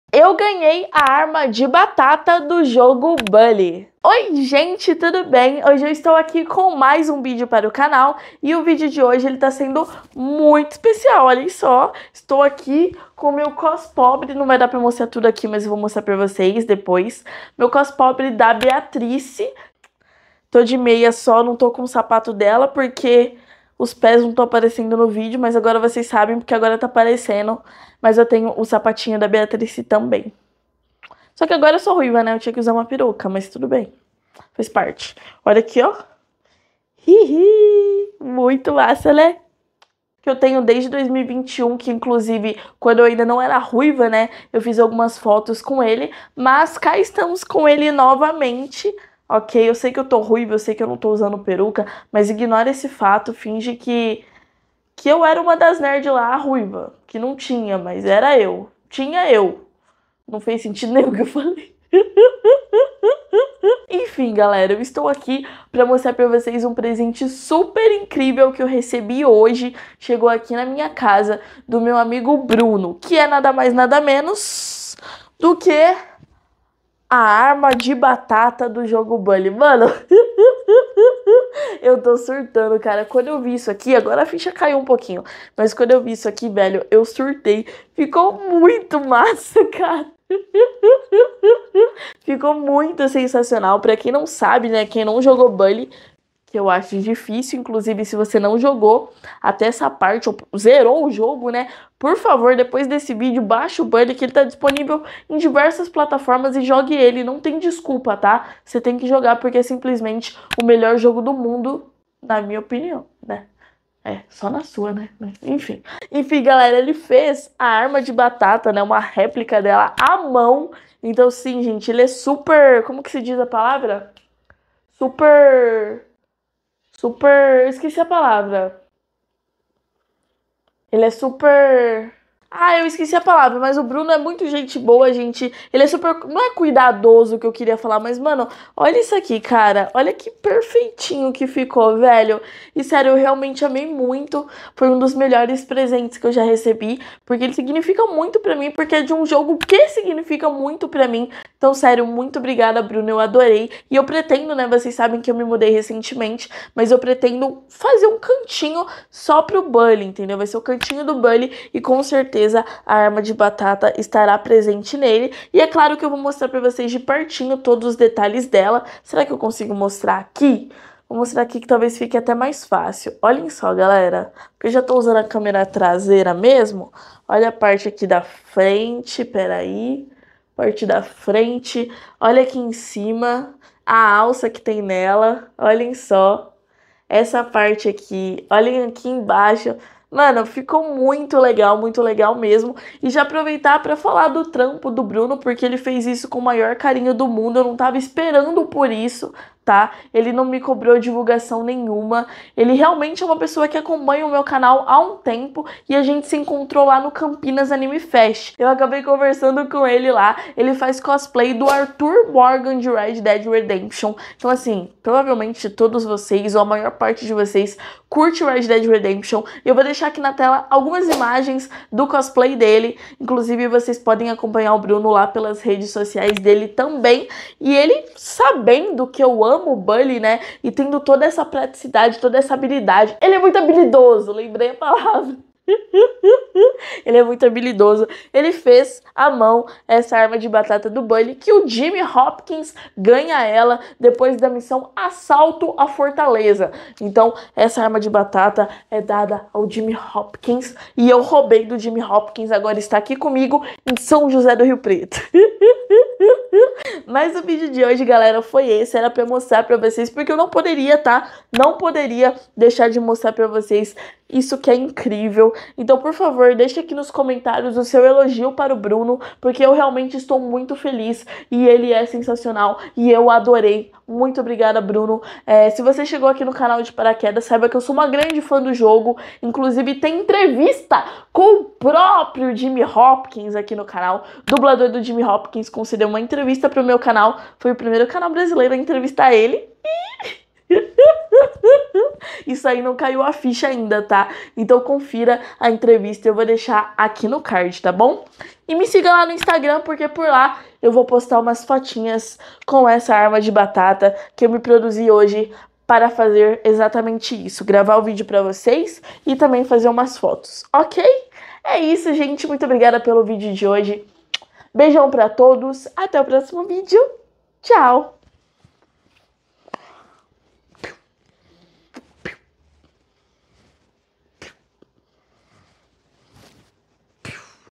GANHEI a SPUD GUN do JIMMY Hopkins do jogo BULLY!
No vídeo de hoje, trouxe para vocês esse presentão que eu ganhei: a arma de batata do Jimmy Hopkins feita à mão, pelo Bruno Barreto!